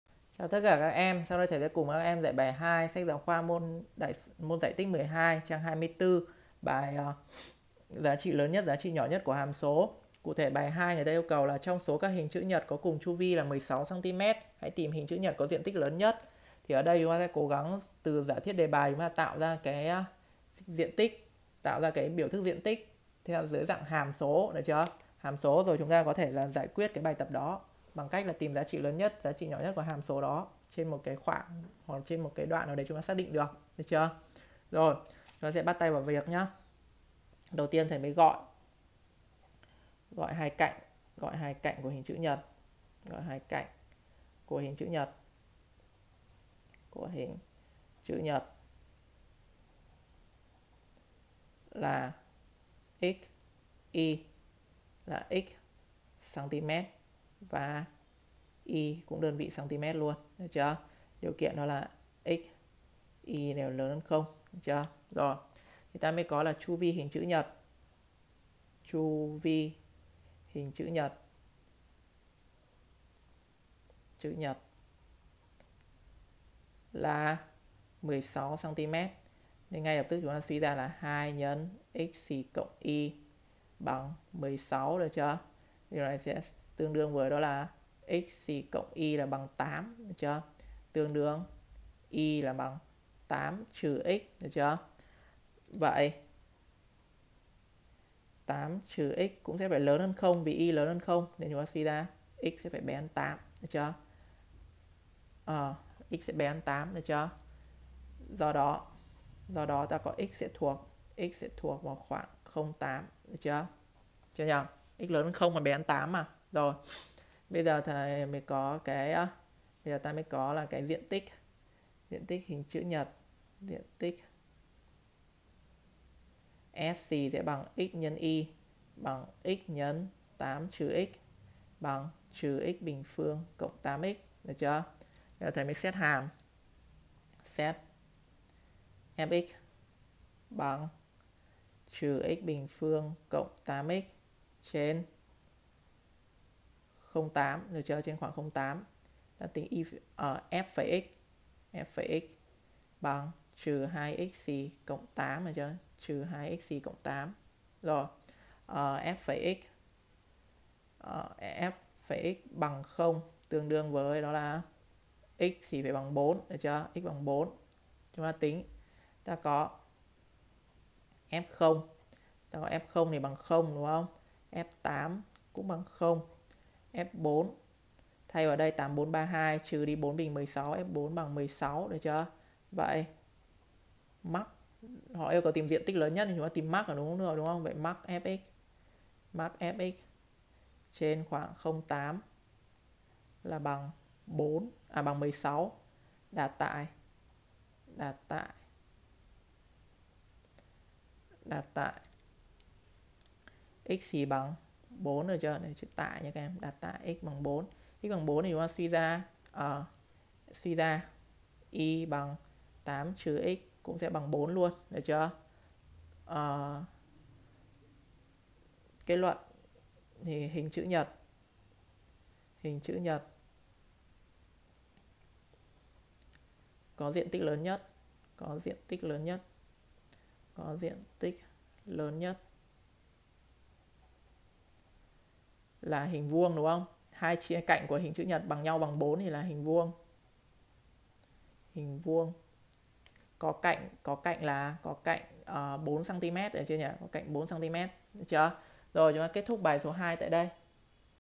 Giải bài 2 trang 23 SGK Giải tích 12
Giải bài 2 trang 24 sách giáo khoa Giải tích 12 với lời giải chi tiết, ngắn gọn nhất sẽ giúp các em nắm bắt các kiến thức cơ bản và nâng cao một cách nhanh nhất
Trong số các hình chữ nhật cùng có chu vi \(16 cm\), hãy tìm hình chữ nhật có diện tích lớn nhất. .